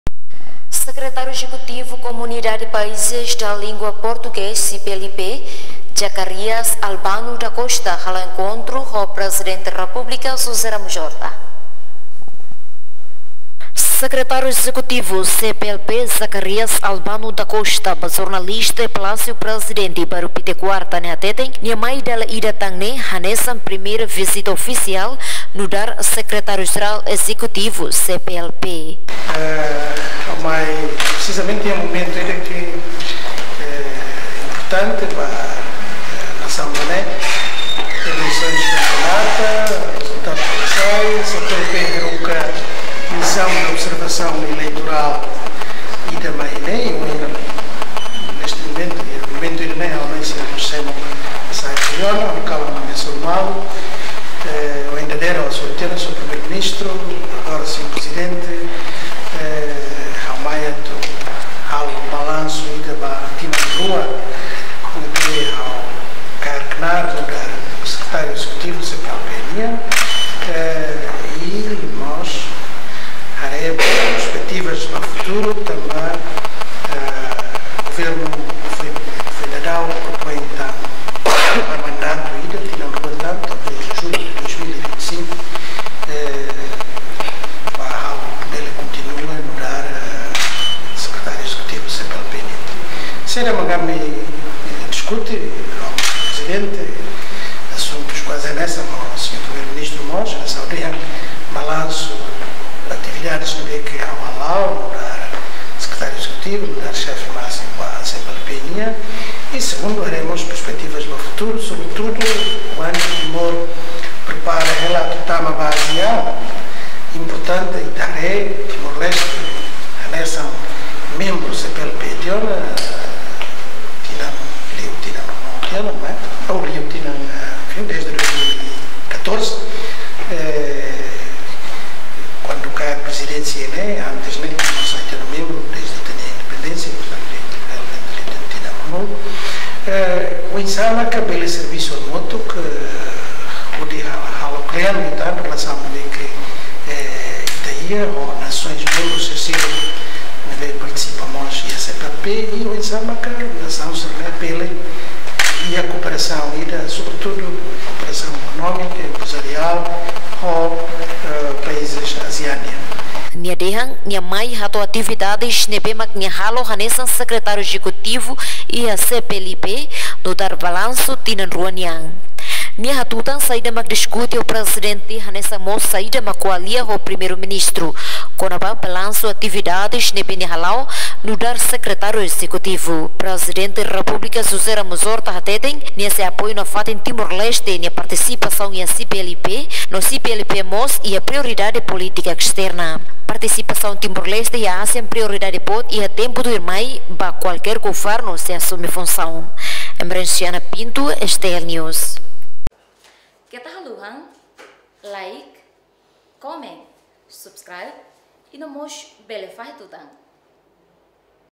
Sekretariu Ezekutivu CPLP Hatoo Progresu Servisu Ba PR Horta
Sekretariu Ezekutivu Comunidade Paizes Da Lingua Portugues, CPLP, Zacarias Albano Da Costa, Halo Enkontru Ho Prezidente Republika Jose Ramos Horta, Hanesan Primeiru Vizita Ofisial Nuudar Sekretariu Jeral Ezekutivu Cplp, Hodi Hatoo Progresu Serbisu Neebe Nia Halao Durante Tinan Rua Nia Laran.